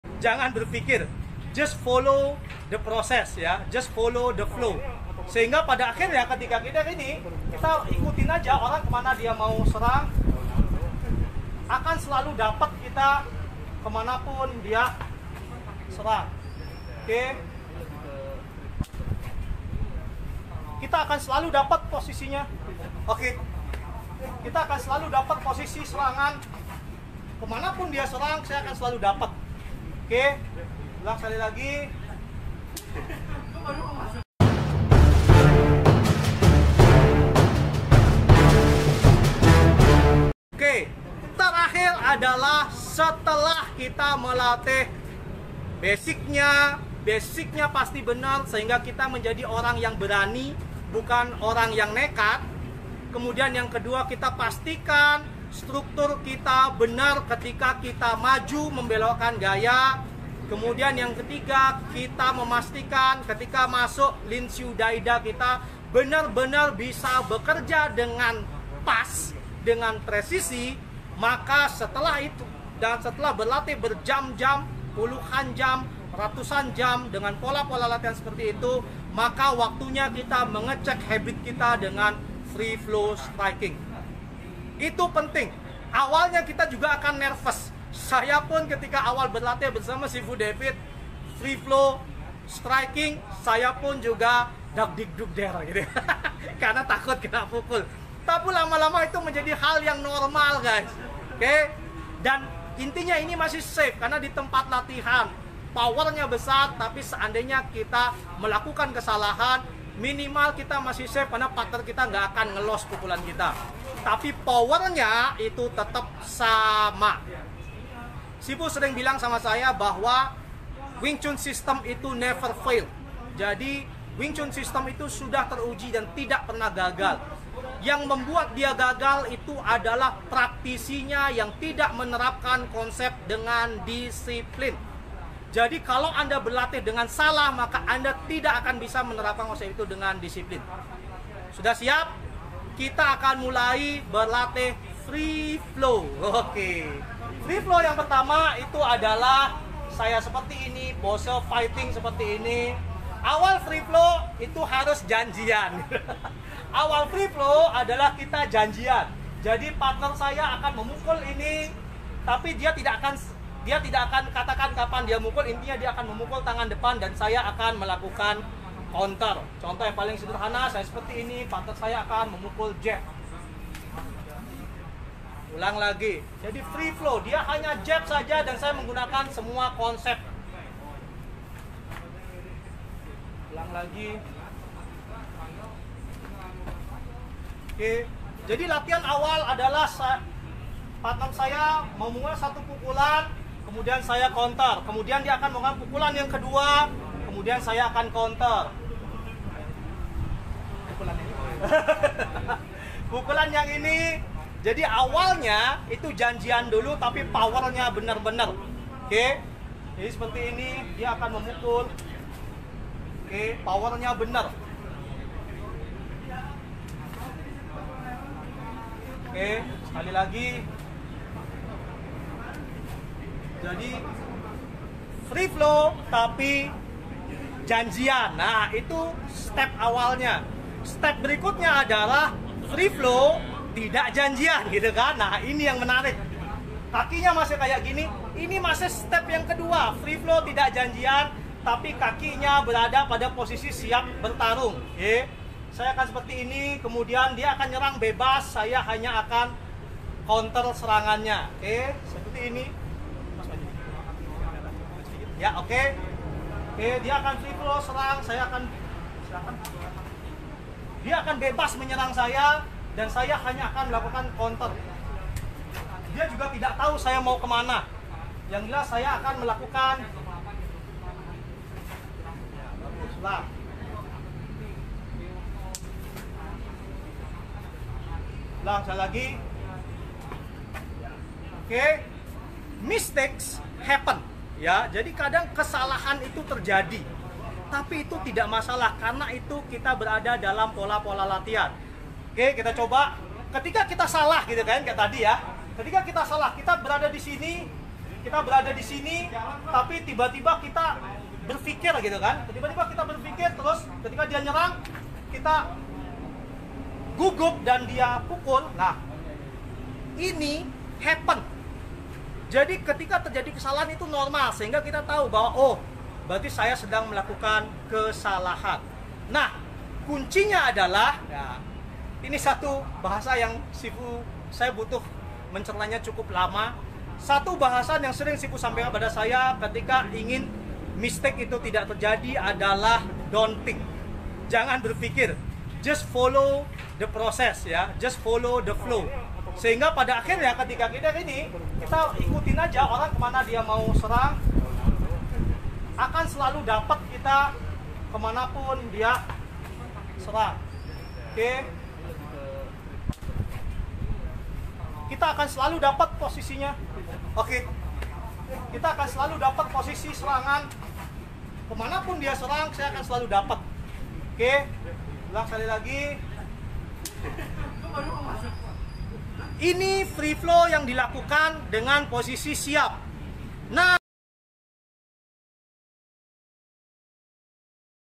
TES SERANGAN BEBAS - WING CHUN REAL FIGHT (MY WSL VING TSUN JOURNEY 33)
(Part 5) Combat Ving Tsun kembali berbagi dalam WSL Wing Chun Journey 33 tentang  Wing Chun Real Fight  Ip Man  yaitu Tes Serangan Bebas sesuai dengan pembelajaran David Peterson Wing Chun dari Wong Shun Leung sesuai materi WSL Ving Tsun Combat Science dari Ip Man Wing Chun.

Combat Ving Tsun Indonesia bersama Sifu Rama Nugraha merupakan salah satu Wing Chun Indonesia aliran Ip Man selalu berkomitmen untuk menjaga kualitas.